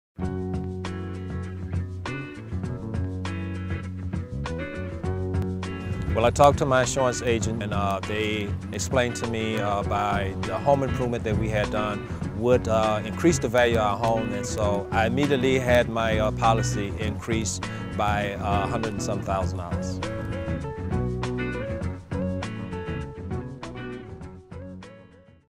Long Siding Increased the Value of Elwood's Home
Elwood explains how investing in Long Siding boosted his home's value.